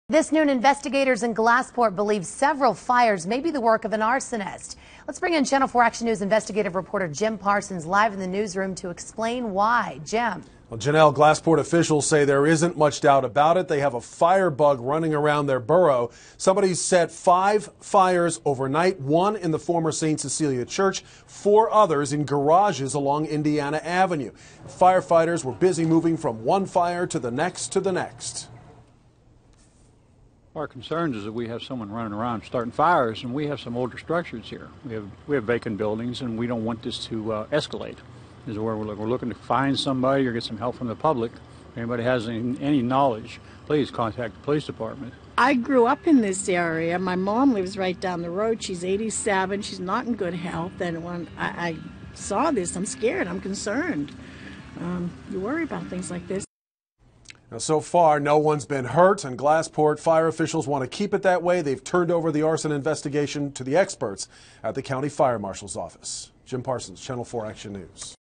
Multiple fires under investigation in Glassport
Investigators believe five fires in Glassport overnight are the work of an arsonist.